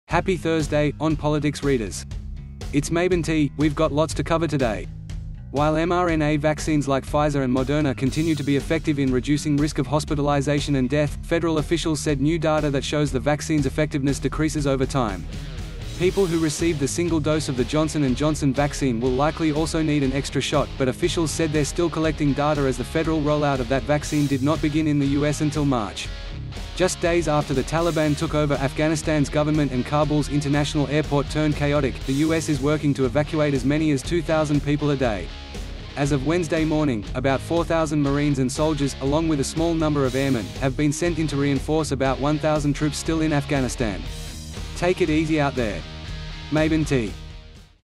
OnPolitics COVID 19 booster shots are coming
Happy Thursday, OnPolitics readers! It's Mabinty, we've got lots to cover today. While mRNA vaccines like Pfizer and Moderna continue to be effective in reducing risk of hospitalization and death, federal officials said new data that shows the vaccines' effectiveness decreases over time. People who received the single-dose of the Johnson & Johnson vaccine will likely also need an extra shot, but officials said they're still collecting data as the federal rollout of that vaccine did not begin in the U. S. until March. Just days after the Taliban took over Afghanistan's government and Kabul's international airport turned chaotic, the U. S. is working to evacuate as many as 2,000 people a day. As of Wednesday morning, about 4,000 Marines and soldiers, along with a small number of airmen, have been sent into reinforce about 1,000 troops still in Afghanistan. Take it easy out there. — Mabinty